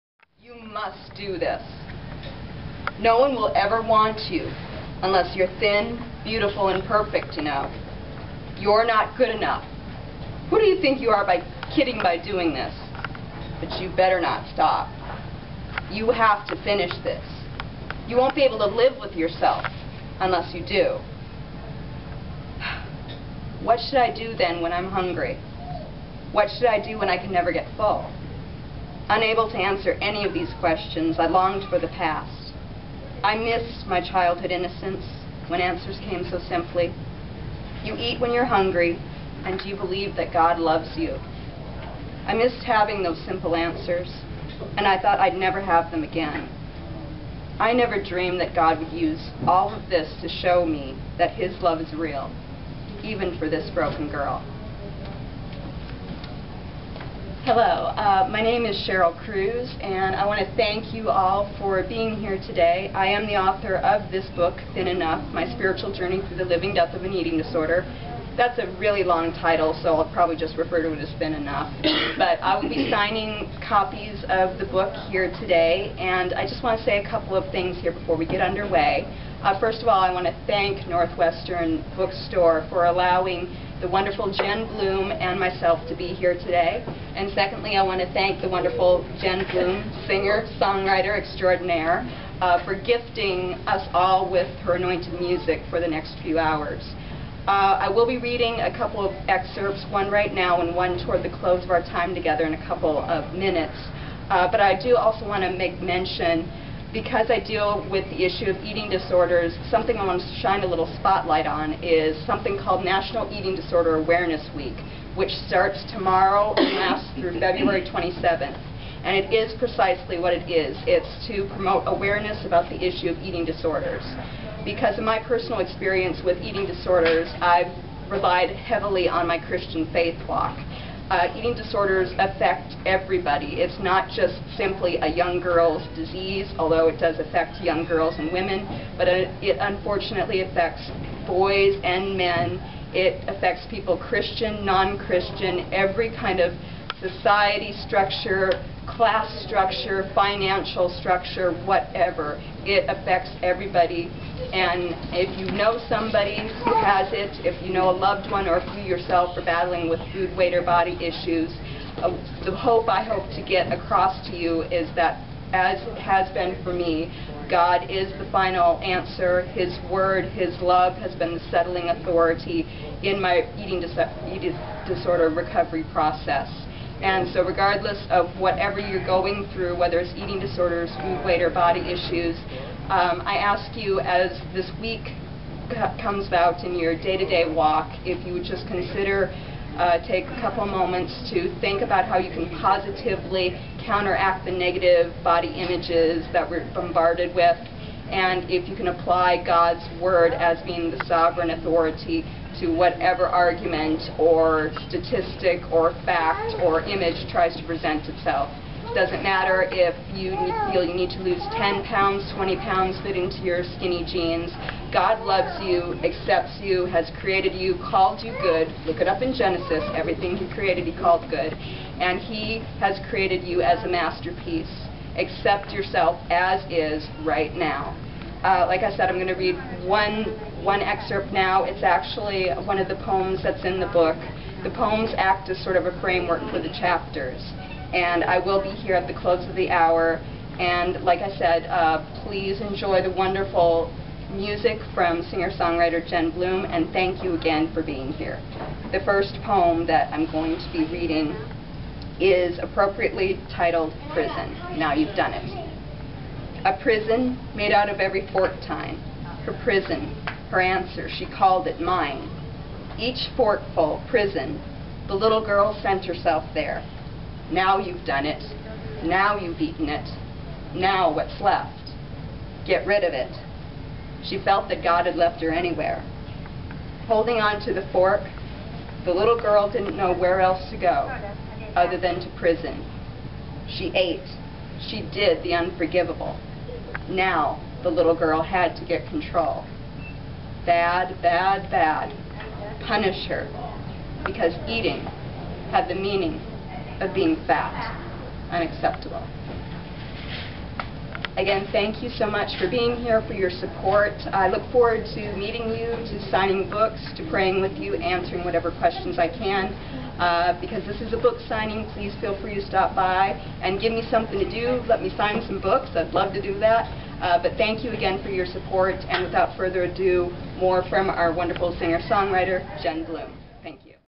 "Thin Enough" Book Introduction
Author/Speaker, Sheryle Cruse introduces excerpts and discusses portions of her book, "Thin Enough: My Spiritual Journey Through the Living Death of an Eating Disorder," at a Northwestern Bookstore book signing, February 20th, 2010.